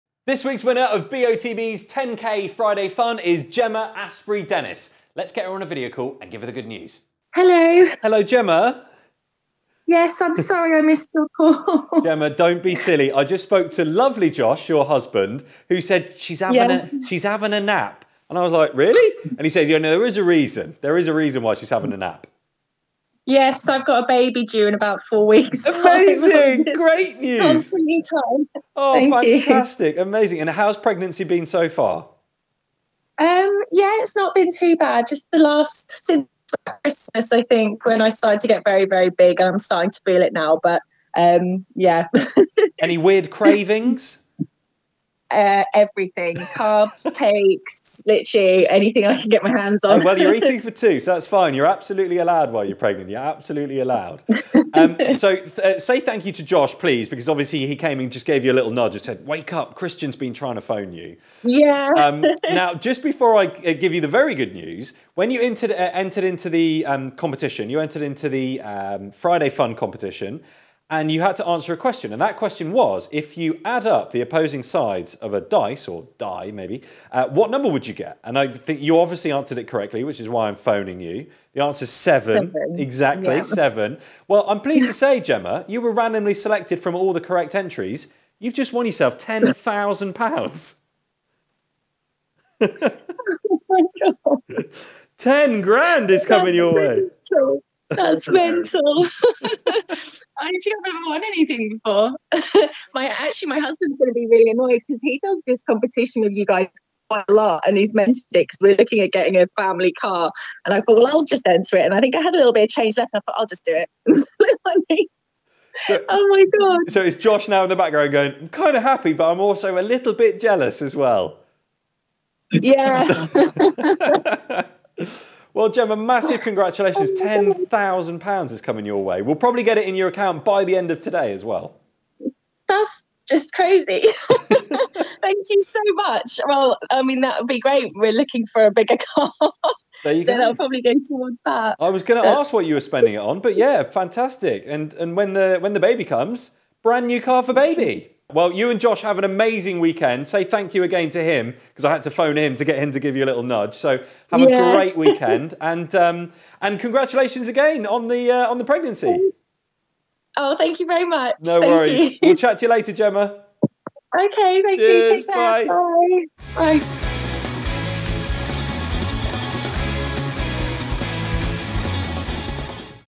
OH BABY, WHAT A WIN! £10,000 BOTB Cash Winner!
Win your Dream Car with BOTB | Enter the BOTB Dream Car & Midweek Lifestyle Competitions from just 50p | Join our list of winners!

Gemma Astbury-Dennis from Norwich is expecting a baby next month, so there couldn't have been a better time for her to scoop a cool £10,000 cash in the Friday Fun competition!

"That's mental", she exclaimed before revealing that she's now planning to spend her winnings on a new family car.

Massive congrats, Gemma, and all the best for the baby!